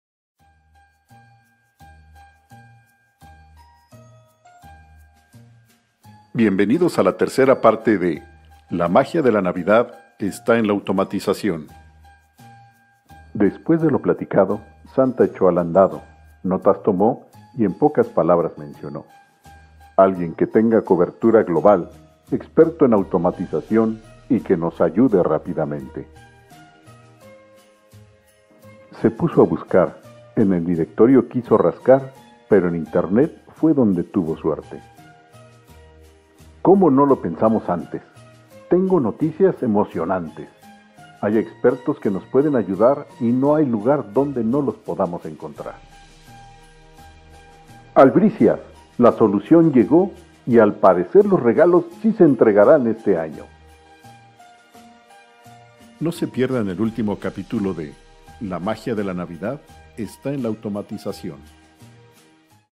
La magia de la navidad esta en la automatización - PT 3
🔄🌐 ¡Bienvenido a la Parte 3 de "La magia de la navidad esta en la automatización"! La trama se complica, revelaciones sorprendentes y giros emocionantes aguardan. Sumérgete más profundo en este fascinante relato de tecnología, magia y cambio. ¡La historia se intensifica! 📚⚙️